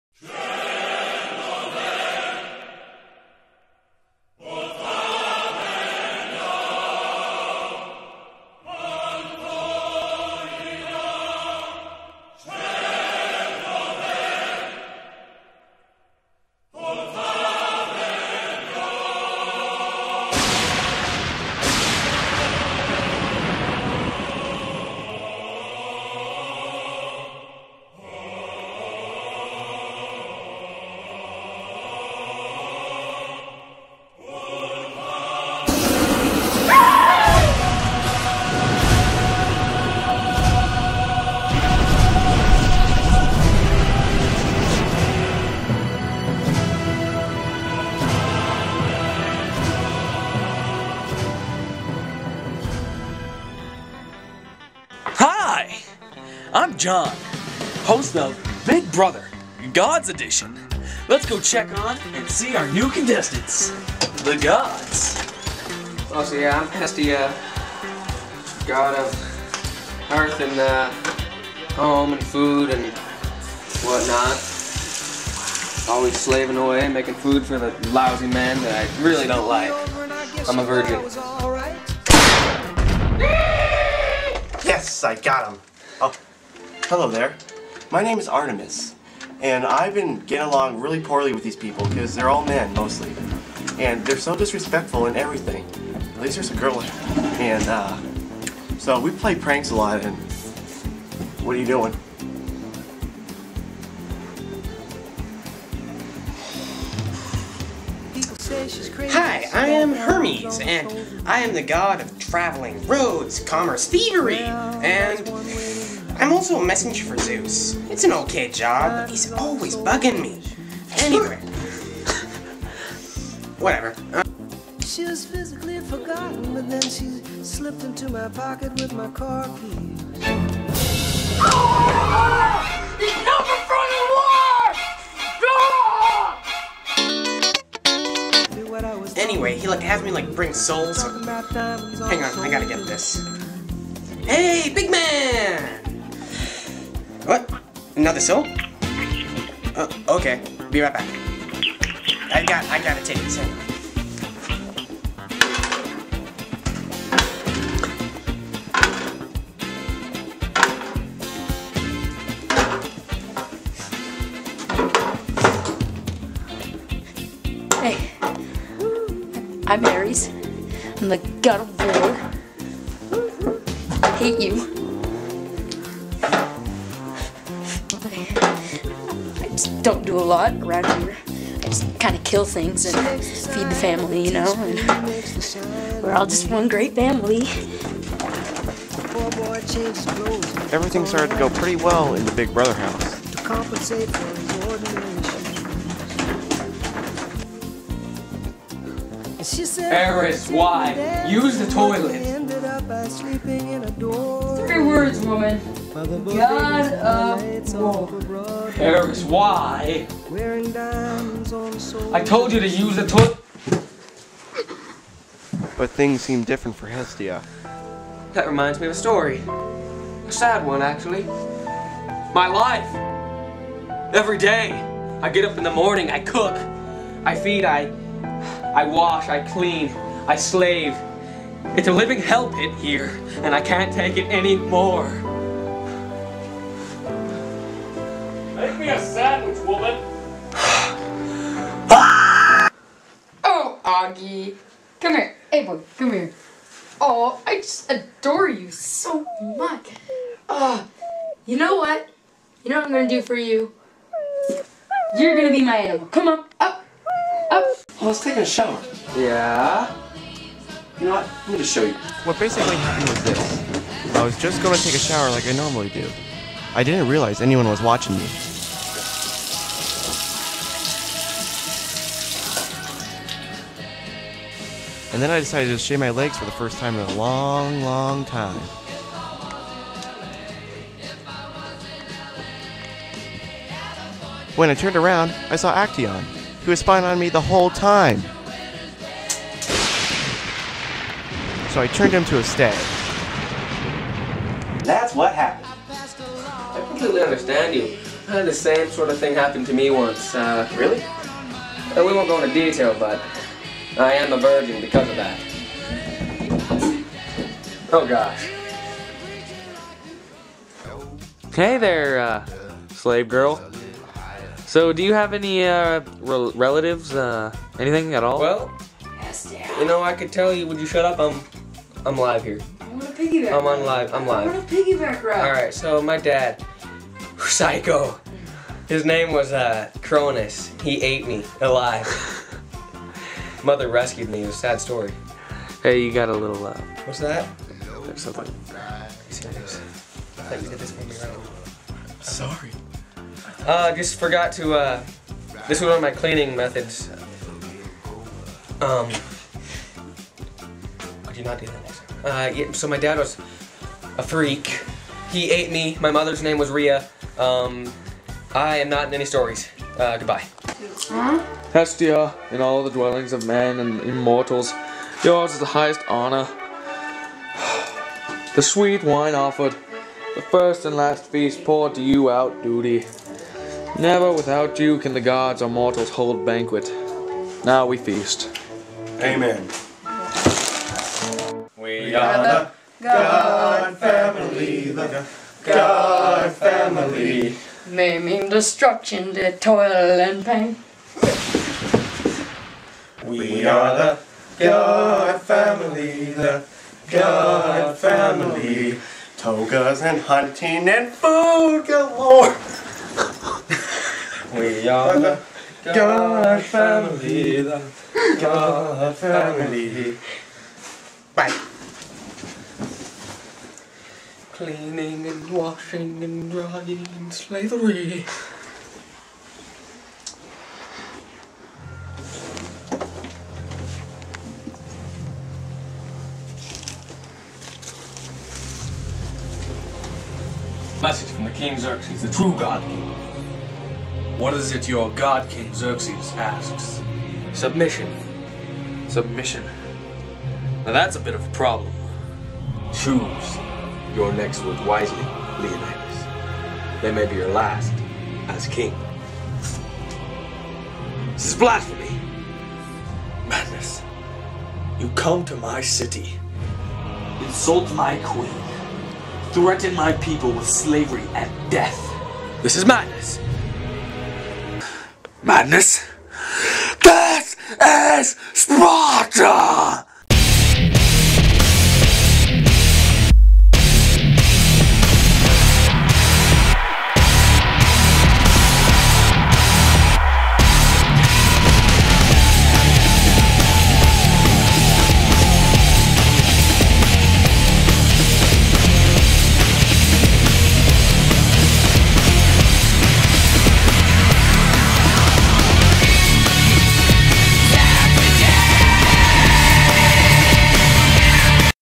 When Gods Collide
This is a story about four gods. There names were Hermes, Hestia, Artimus, and Ares. They were chosen to be on the next season of big brother. They had to live in one house and live together as roomates. Artimus would gather the food by hunting for the housmates, and Hestia would cook for them. Hermes would go out and be a messenger for Zues, and Ares really did nothing in the big brother house. He usually maimed on other people and plotted to kill everyone. Anyways, watch the drama unfold and the housemates become frustrated with each other and stories are told to our camera man. They also gather together and sing a song at the end.

Meanwhile, the messenger of King Xerxes askes King Leonidas for submission. Watch what happens in our surprise clip at the end of the movie.